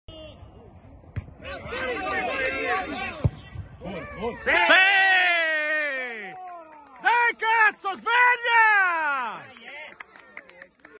Lombardia - Amatori UISP Calcio a 11 Provinciali Monza Brianza A G18 - Excelsior 1995 vs Leoni...
I video di Tuttocampo: Lombardia - Amatori UISP Calcio a 11 Provinciali Monza Brianza - Girone A - Giornata 18 - Excelsior 1995 vs Leoni Arcore